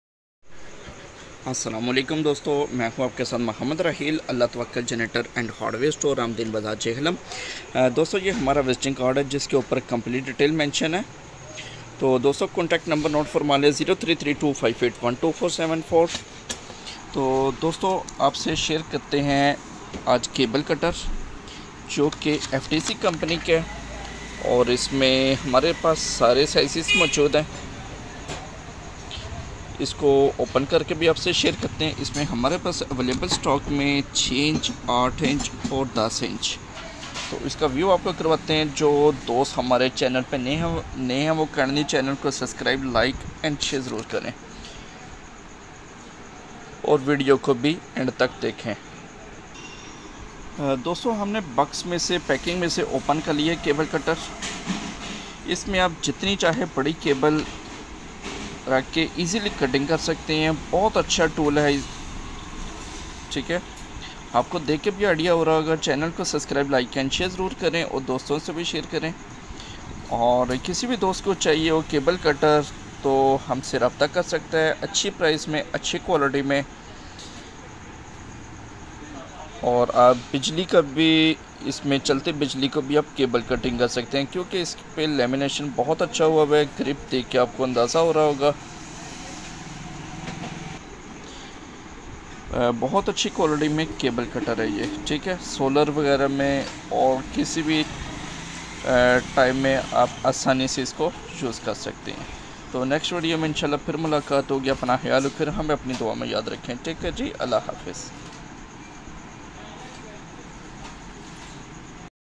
Precision Ftc Cable Cutter Cutting Pliers 6" 8" 10" Heavy duty for Electrical tool Wire rope
Professional Cable Cutter
Precision forging of CR-V Chromium -Vanadium steel is adopte It is to Break steel wire in Seconds by cutting tightly.
The maximum shear is 4 mm for low carbon steel and 2.2 mm for medium carbon steel.
Feature:
Durable-make.It's very precise and saving effort for cutting vinyl coated or copper cable.
Cutting edges are sharp and precision machined to appropriate angle to cut the cables with ease.
Cutting Diameter : Ø 9 mm
Material:
Application: Cutting  vinyl coated or copper cable; Electrician; DIY
OEM、ODM: Available
Don't cut steel or ACRS.